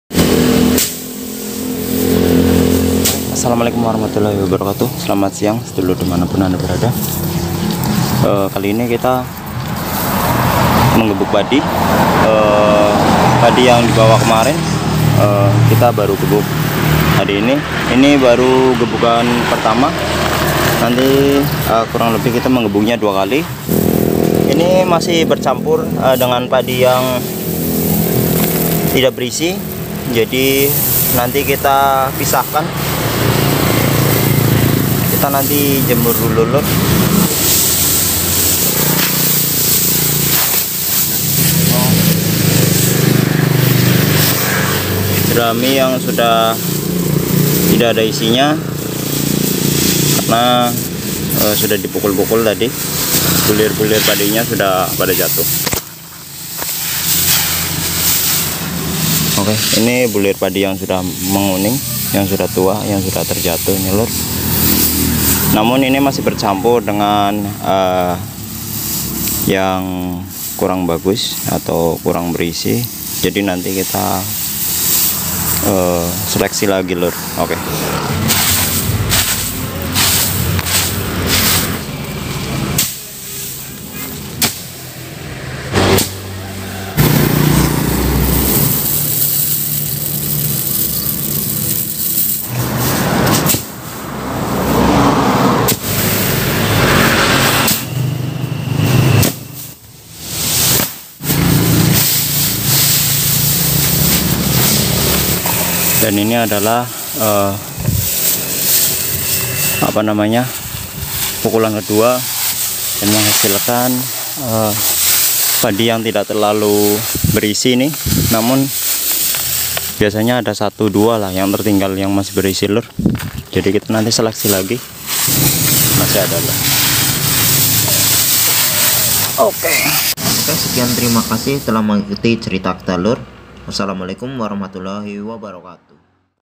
MERONTOKAN PADI CARA MANUAL@CERITAKITALUR
Cara merontokan padi secara manual memang membutuhkan waktu yang terbilang lama,semua kembali pada masing masing orang#viralvideo pertanian petani padi panen panenpadi cara @CERITAKITALUR